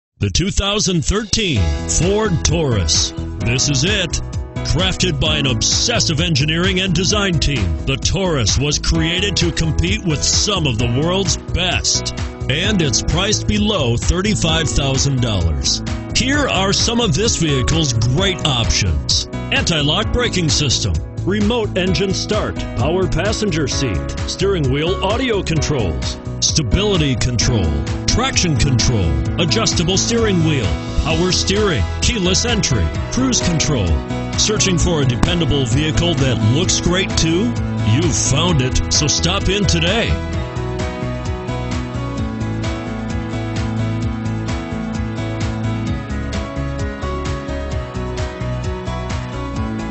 2013 FORD TAURUS Taylor, MI 206077
2013 FORD TAURUS Taylor, MI
Stock# 206077

You'll love this 2013 Ford Taurus. This is a car you'll want to take home. With 1 miles, it features Automatic transmission and an exterior color of Kodiak Brown Metallic. Call us and be the first to open the car door today!